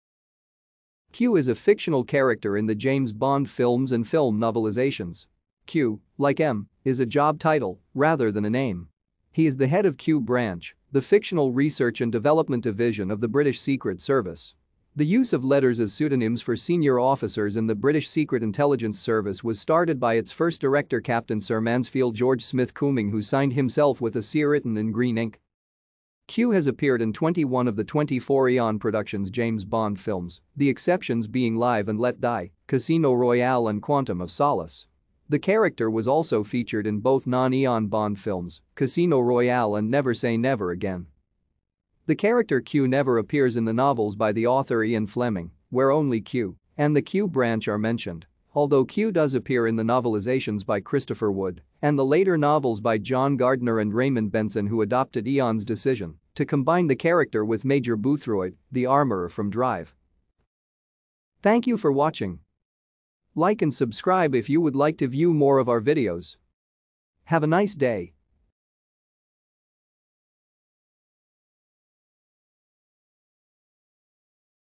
Q (James Bond)
Q is a fictional character in the James Bond s and  novelisations. Q, like M, is a job title rather than a name. He is the head of Q Branch, the fictional research and development division of the British Secret Service. The use of letters as pseudonyms for senior officers in the British Secret Intelligence Service was started by its first director Captain Sir Mansfield George Smith-Cumming  who signed himself with a C written in green ink.